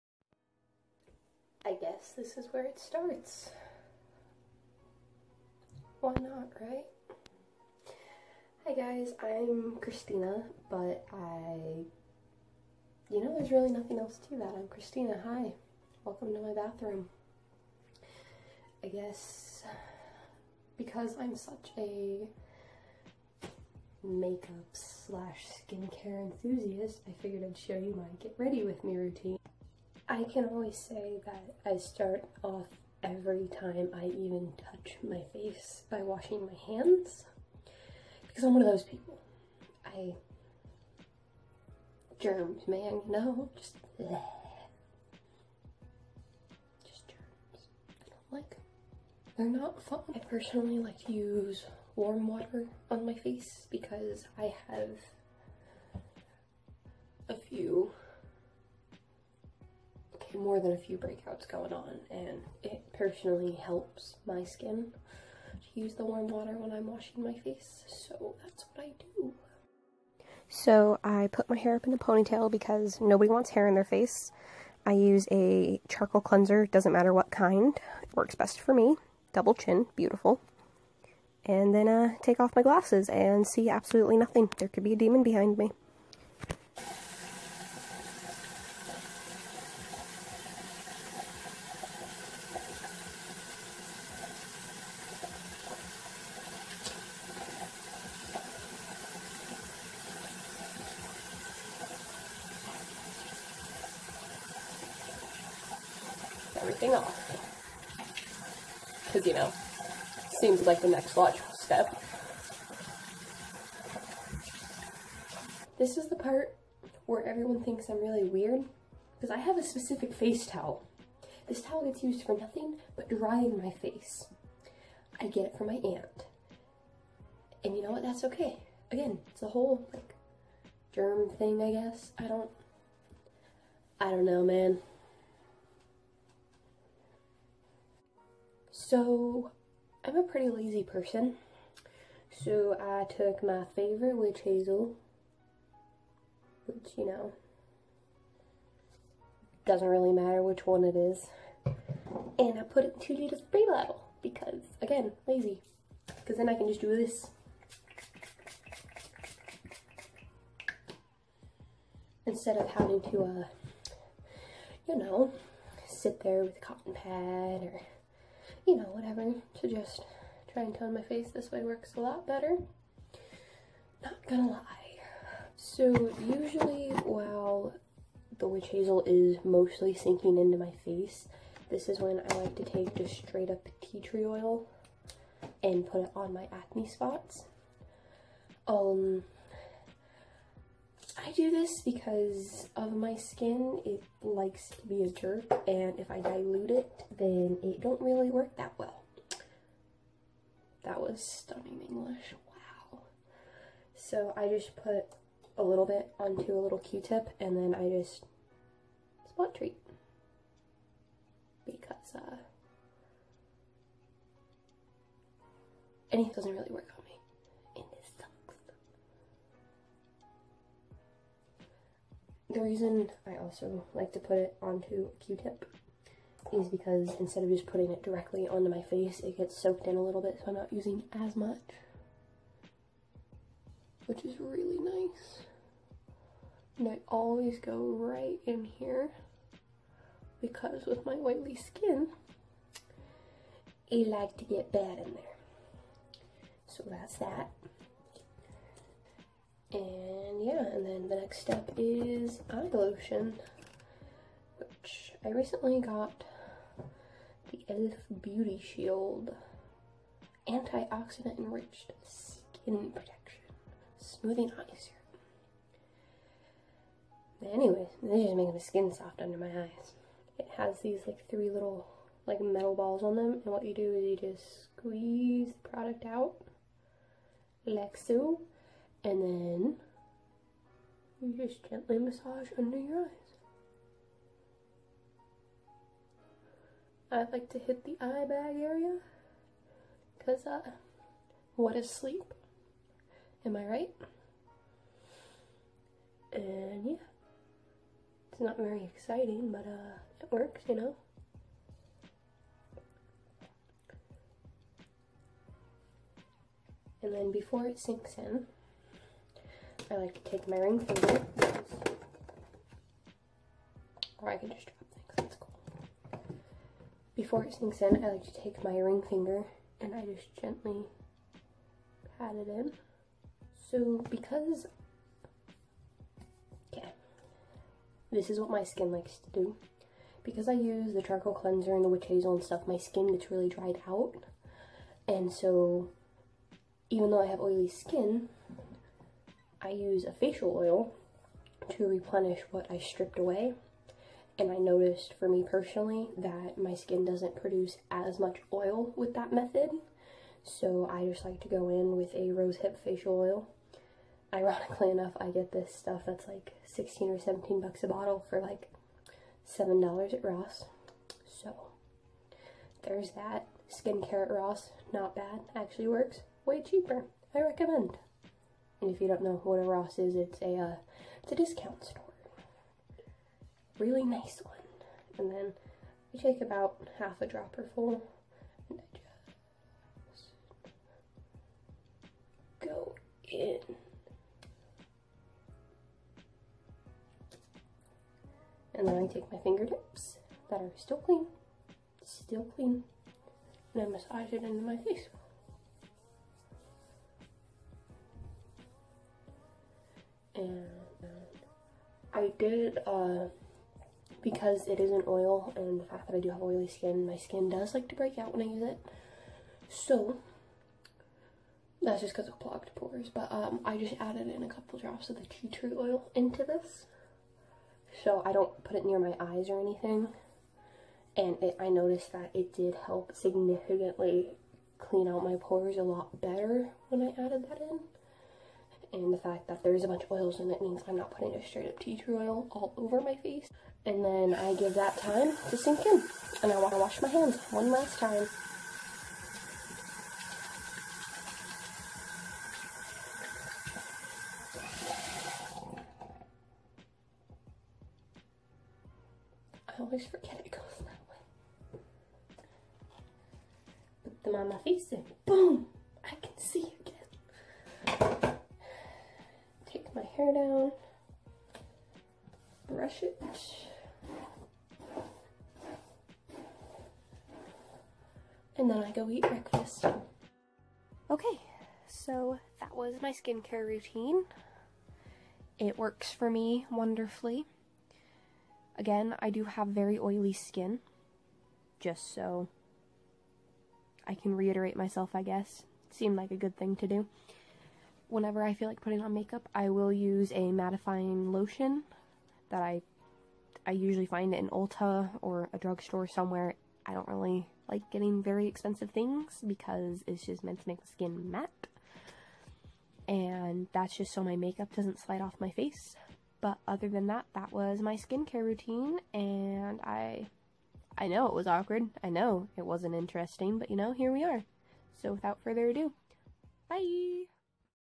My Skincare Routine for oily skin
My name is Kristina and I have highly oily skin. I'm also very awkward and camera shy - ish.

Products Used:
Water
Equate Beauty Charcoal Facial Cleanser
Thayers Witch Hazel with aloe vera and rose petals
Morgan Miller rose hip facial oil